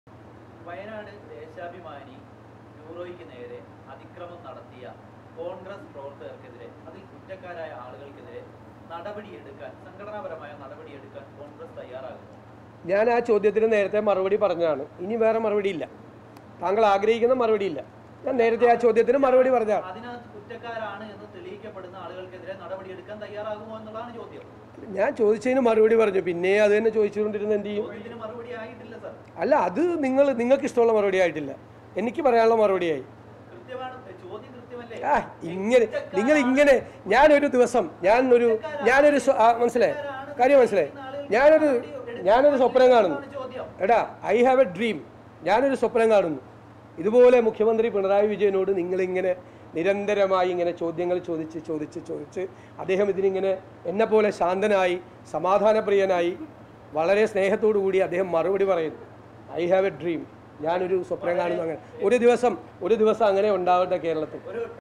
'എന്നെപ്പോലെ ശാന്തനായി, സ്നേഹത്തോടെ മുഖ്യമന്ത്രി ചോദ്യങ്ങൾക്ക് മറുപടി പറയുമെന്ന് സ്വപ്നമുണ്ടെനിക്ക്'
'എന്നെപ്പോലെ ശാന്തനായി, സമാധാനപ്രിയനായി, സ്നേഹത്തോട് കൂടി നിങ്ങളുടെ ചോദ്യങ്ങൾക്ക് മുഖ്യമന്ത്രി മറുപടി പറയുന്ന ഒരു ദിവസം കേരളത്തില്‍ ഉണ്ടാകട്ടെയെന്ന് ഞാൻ സ്വപ്നം കാണുന്നു'വെന്ന് വി ഡി സതീശൻ.

Happy viewing!